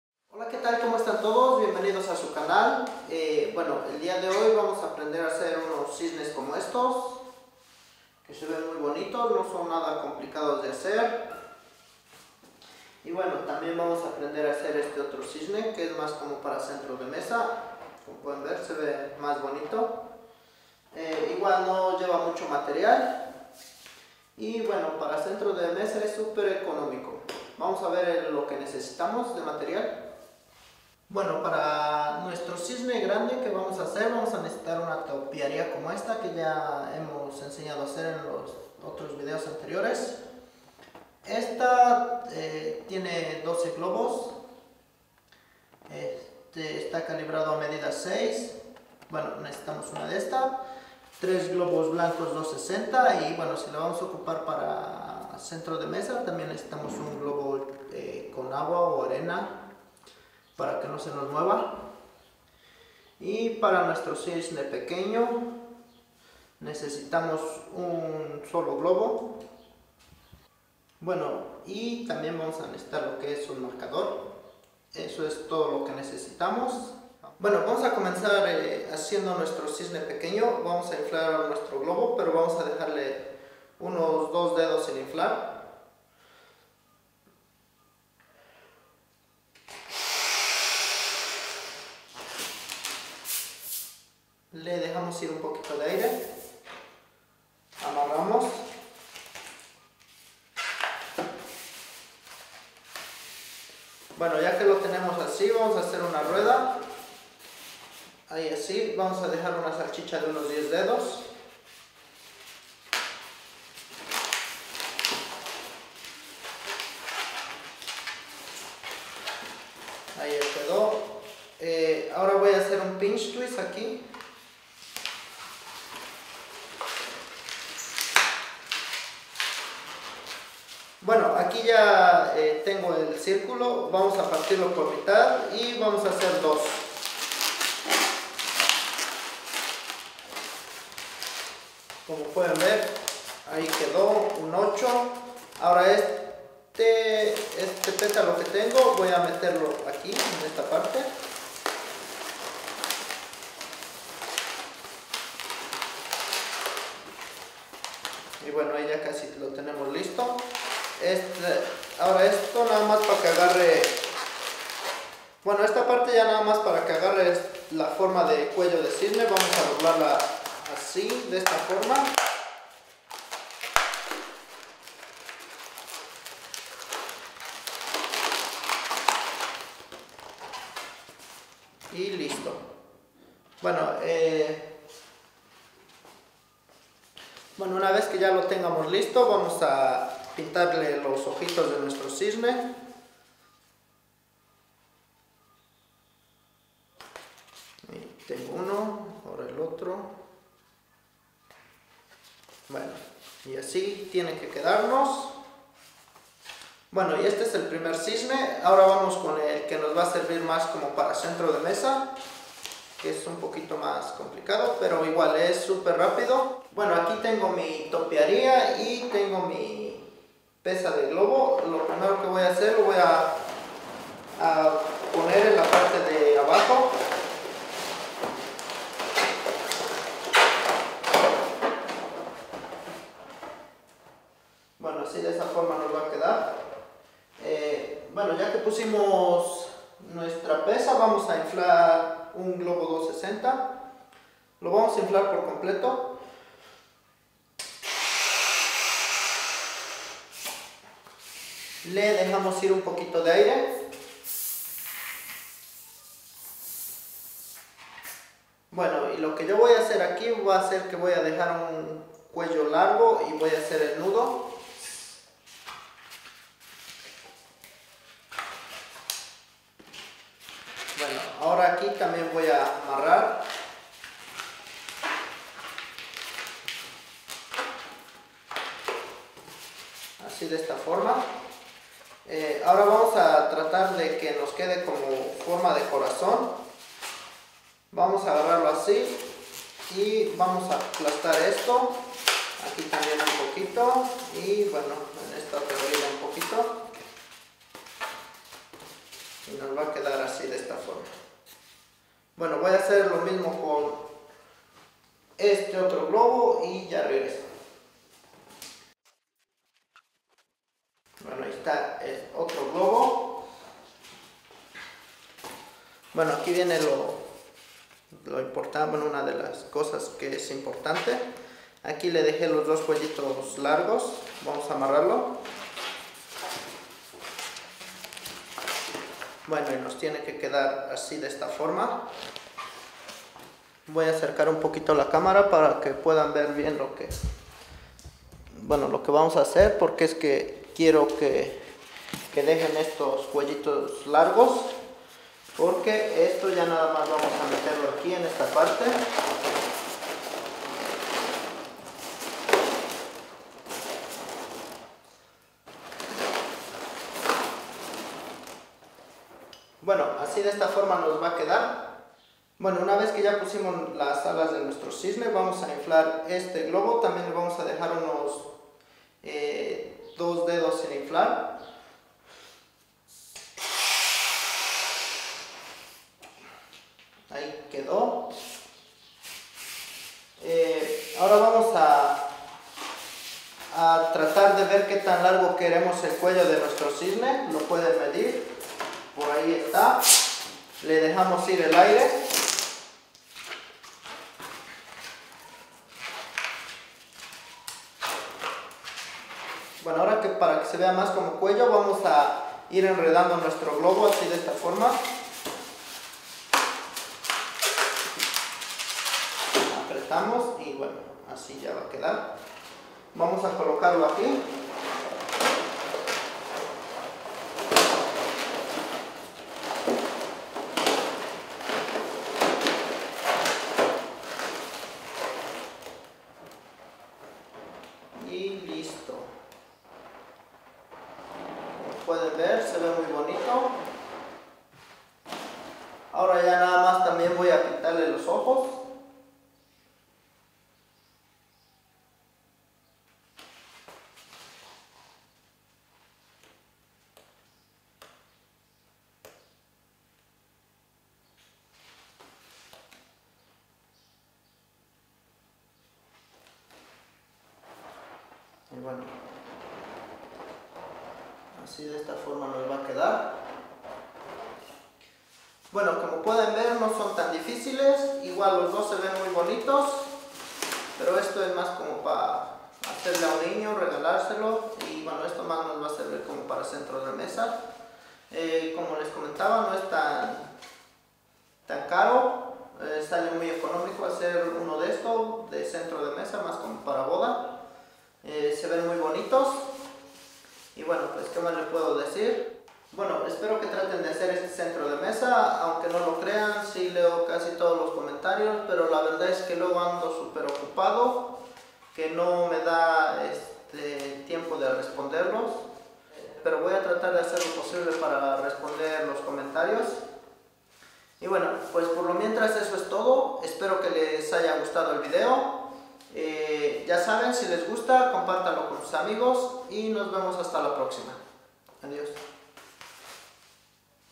Como hacer un cisne con globos de dos formas diferentes!! # 119
Hola en este video haremos un cisne con globos de dos formas diferentes, especial para centro de mesa espero sea de su agrado!!

                            Materiales
1 topiaria color blanco calibrado a # 6
3 globos color blanco # 260
1 globo con agua
1 globo color azul
1 marcador color negro